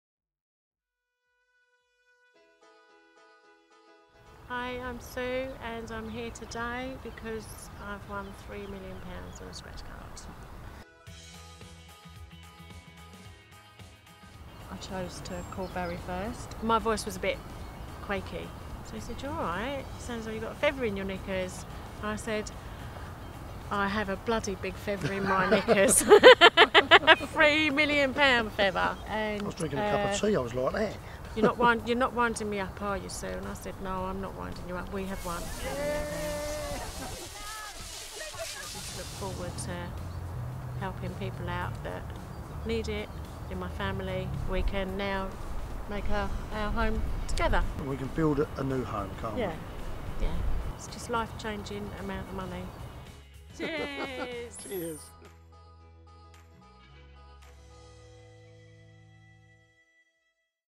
Essex mum on cloud 9 after £3M win
Meet Susan Richards from Rochford, Essex and discover what it feels like to win £3M on a National Lottery GameStore Scratchcard.